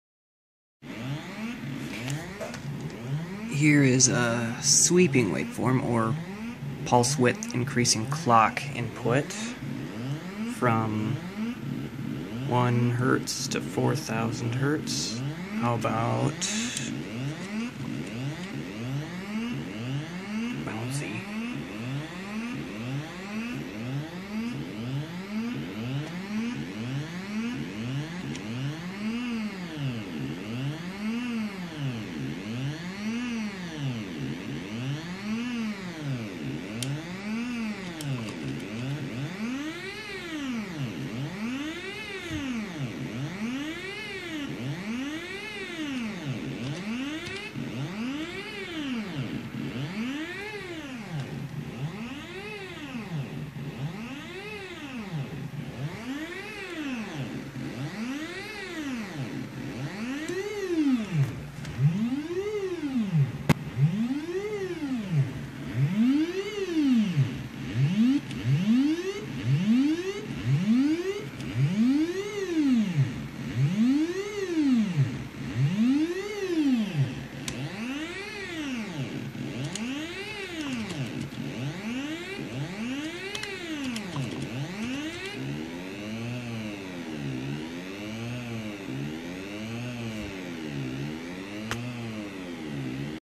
Sweeping Demo
Shift Register Module, Early prototype: Sweeping Demo

This demo shows how the module accepts a clock rate that is modulating in pulse width. First I show it sweeping upward and then I show it bouncing up and down.